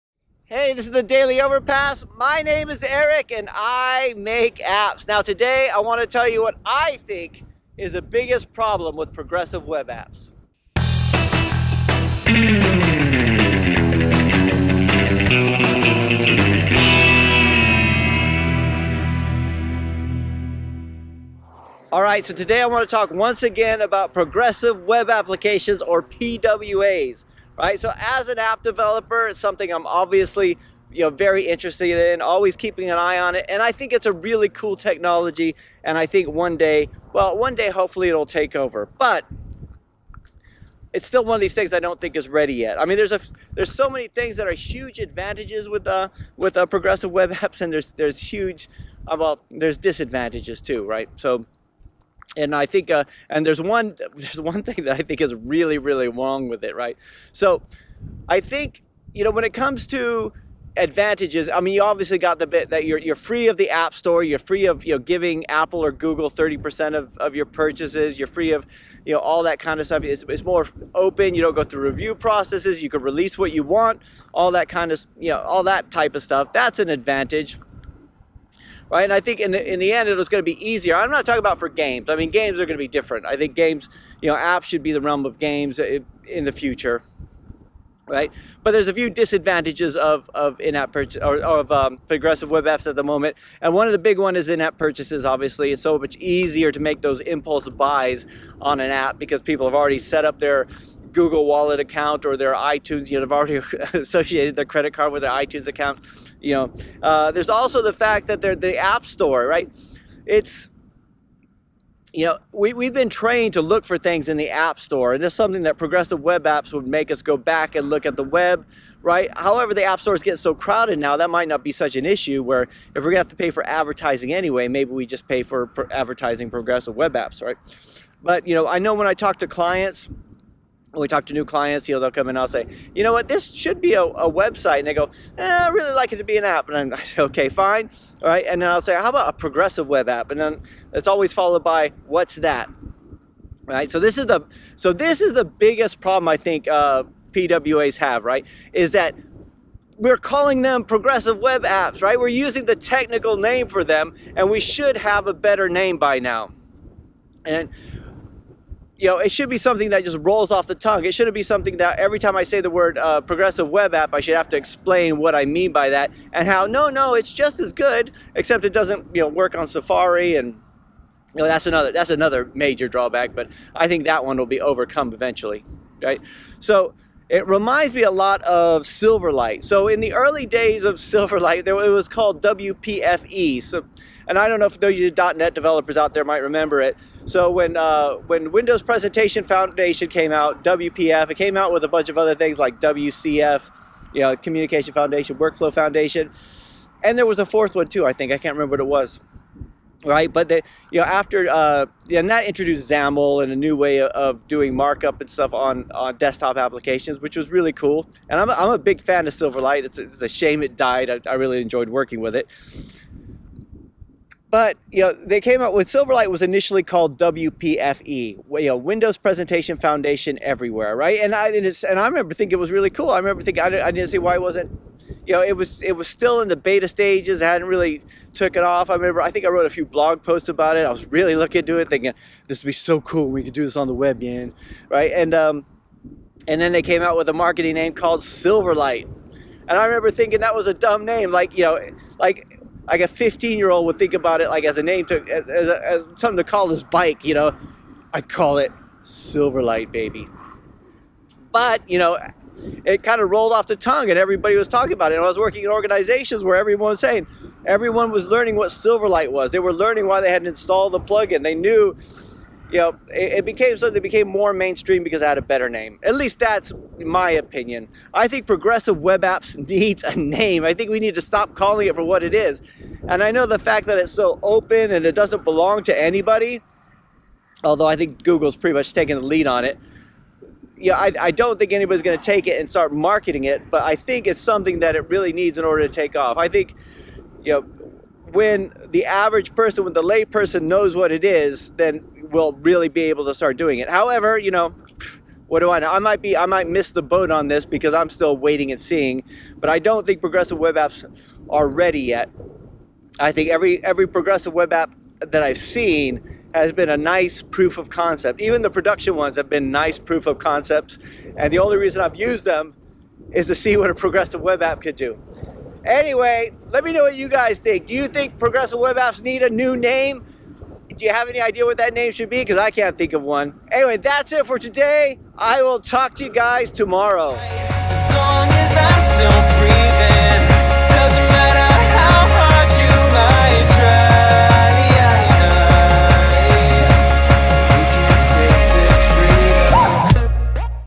The Biggest Problem with Progressive Web Apps (PWAs)
The Biggest Problem with Progressive Web Apps (PWAs) - The Daily Overpass #247

I'm excited about Progressive Web Apps.  Except, I hate calling them "Progressive web Apps!"

So...

Or...